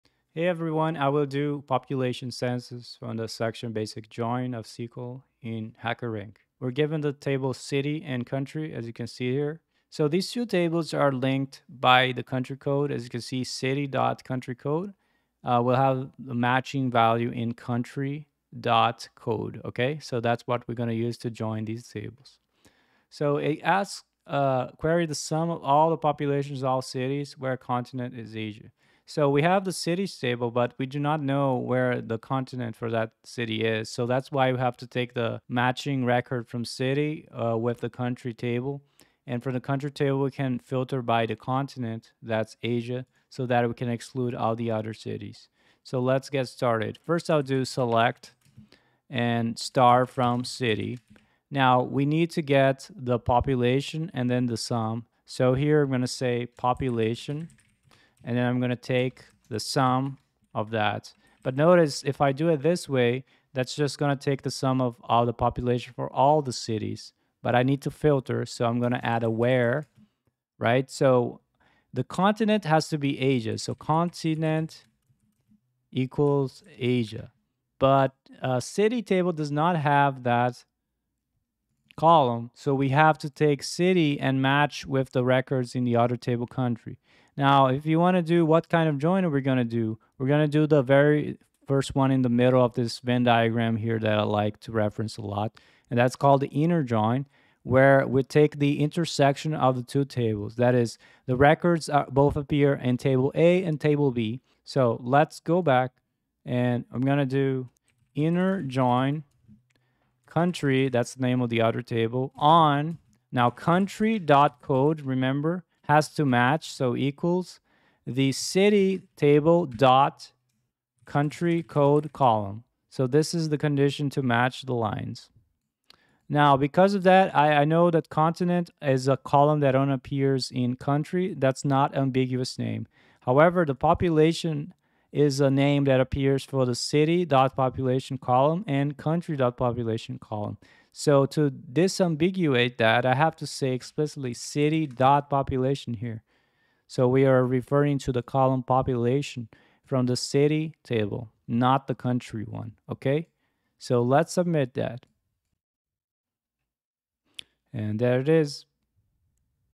Population Census | SQL Basic Join | HackerRank Solution
A lesson that teaches you how to solve the following problem from the SQL section in HackerRank.

Learn: Building a News Blog Web App with Next.js and Express